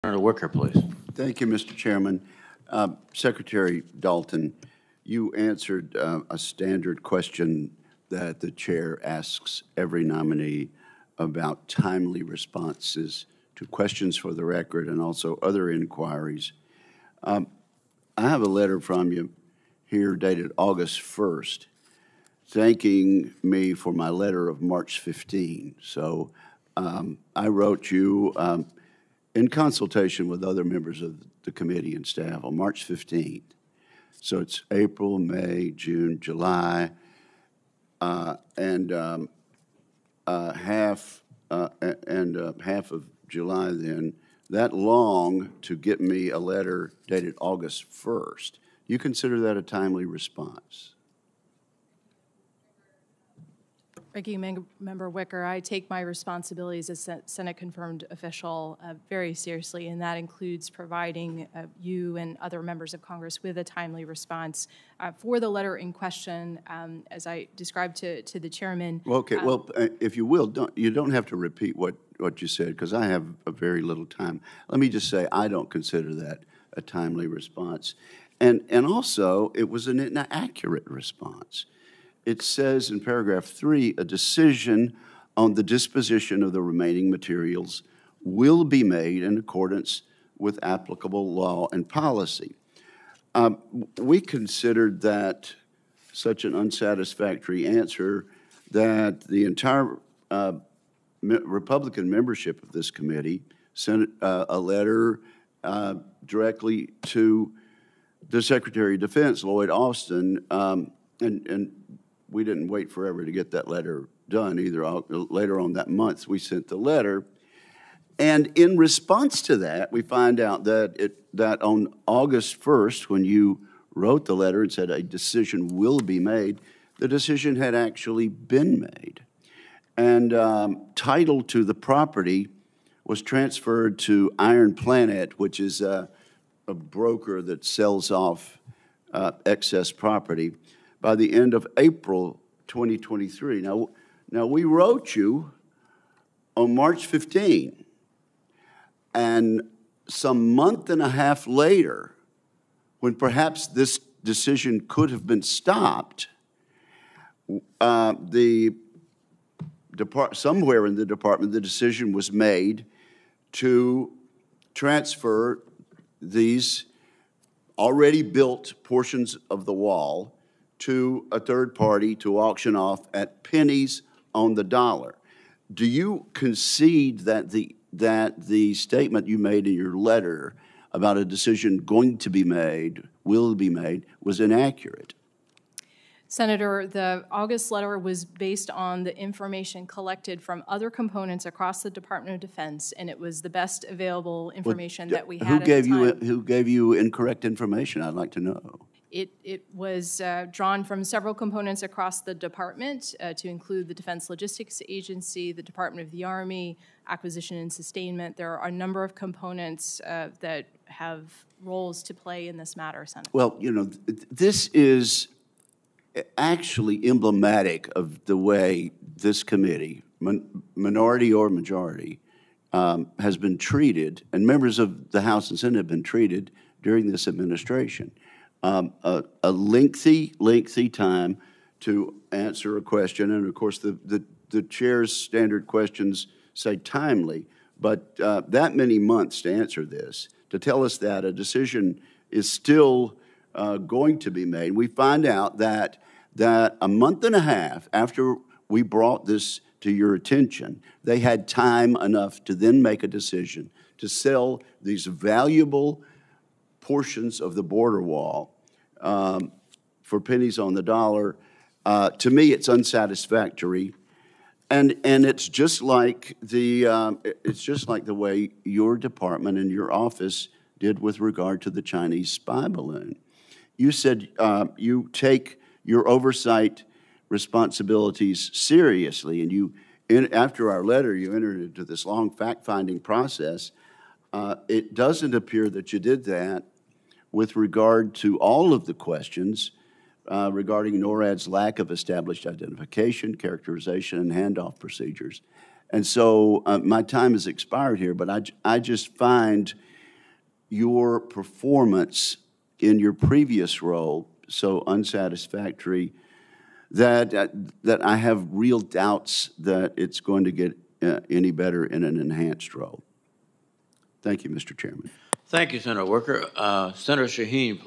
'It Was An Inaccurate Response': Roger Wicker Grills Biden Defense Nom Over Border Material Accounts
During a Senate Armed Services Committee hearing, Sen. Roger Wicker (R-MS) questioned nominees about border wall materials, and the expedience of responses from the nominees previously to the committee.